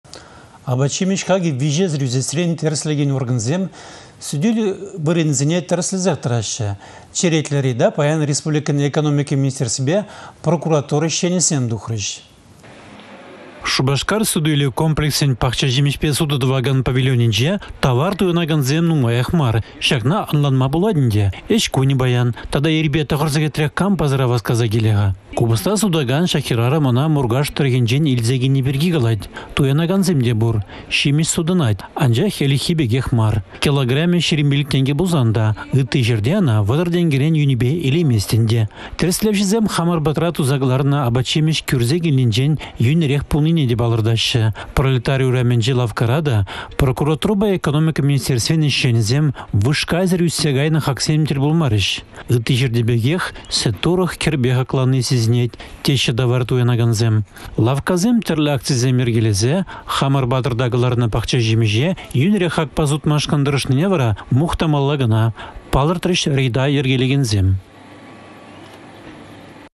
Хаксем ытлашши ÿстермелле март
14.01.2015
Апат-çимĕç хакĕ виçесĕр ÿсесрен тĕрĕслекен органсем суту-илÿ вырăнĕсене тĕрĕслесех тăраççĕ. Черетлĕ Рейда паян республикăн экономика министерствипе прокуратурин ĕçченĕсем тухрĕç.